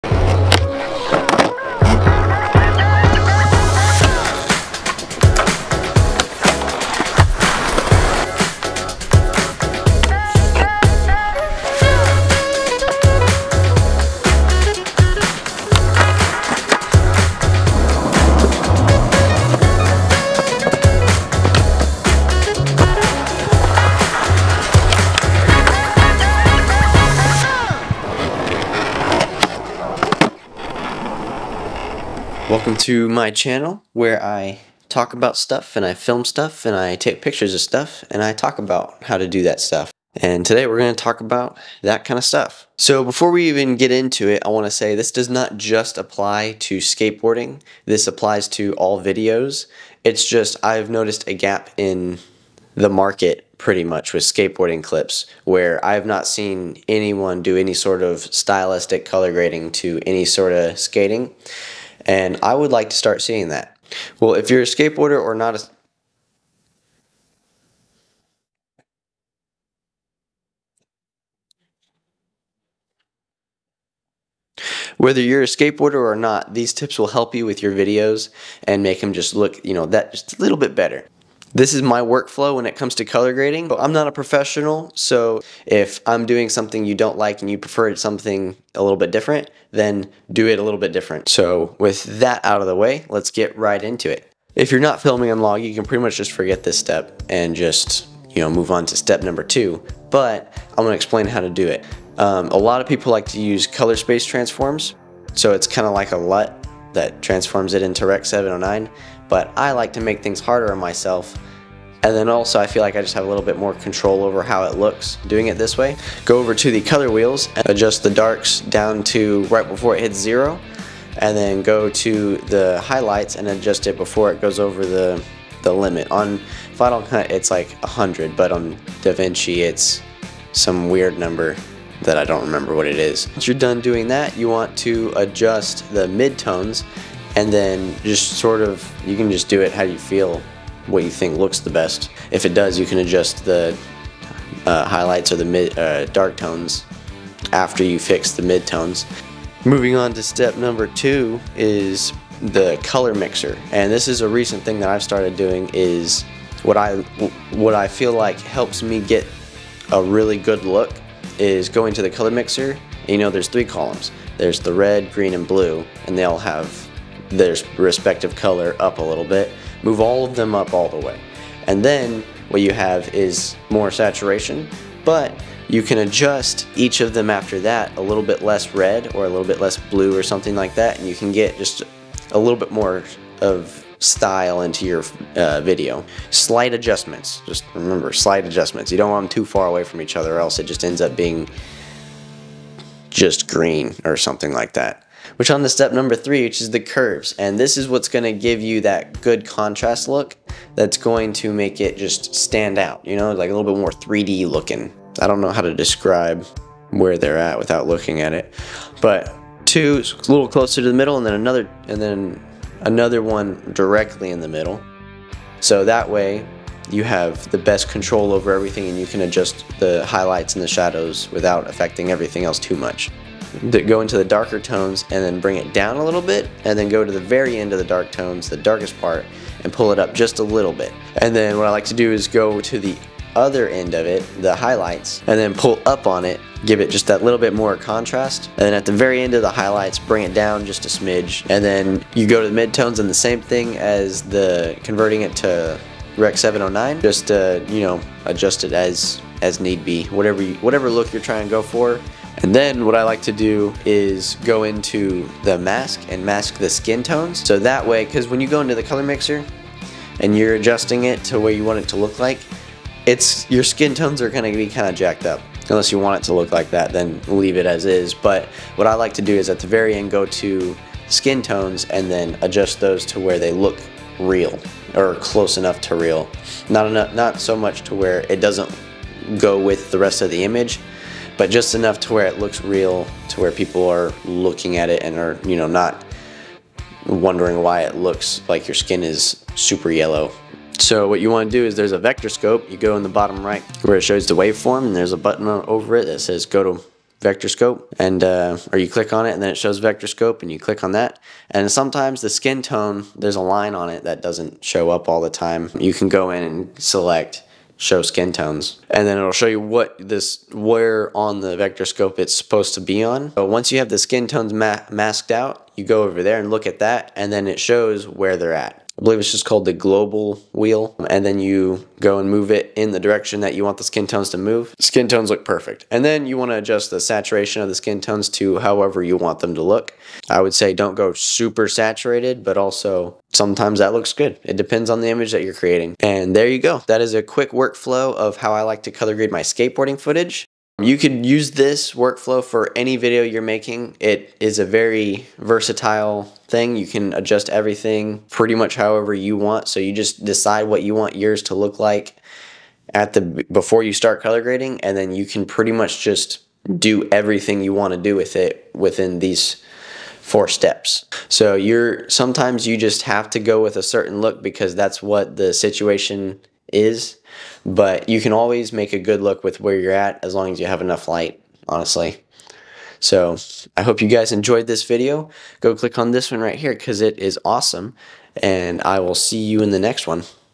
How to COLOR GRADE YOUR SKATE VIDEO to be CINEMATIC in DaVinci Resolve
In this tutorial, I will show you how to make your skate video look like a movie using DaVinci Resolve. Learn the basics of editing and color grading to take your footage to the next level.
My Camera
Canon RF 50mm 1.8 lens
Canon RF 16mm 2.8 lens
My Microphone
Magic arm